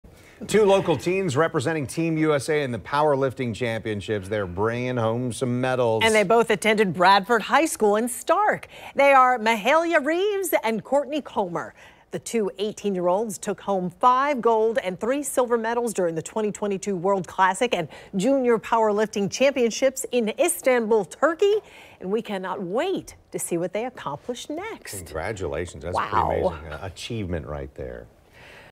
Local teens representing Team USA win gold, silver medals during 2022 Powerlifting Championships
Mahailya Reeves and Courtney Comer — two 18-year-olds from Bradford High School in Starke — took five Gold medals and three Silver medals home during the 2022 World Classic & Equipped Sub-Junior & Junior Powerlifting Championships in Istanbul, Turkey on September 04. Mahailya Reeves, who just graduated from Bradford High School, won four gold medals — placing first overall for the 84 Kg+ class at the 2022 Junior World Championships. Courtney, a senior at Bradford High School, won one gold and two silver medals placing second overall for the 76Kg class at the 2022 Sub-Junior World Championships! Courtney went 5/9 totaling 427.5kg/942lb finishing in second place in the 76kg sub-junior class.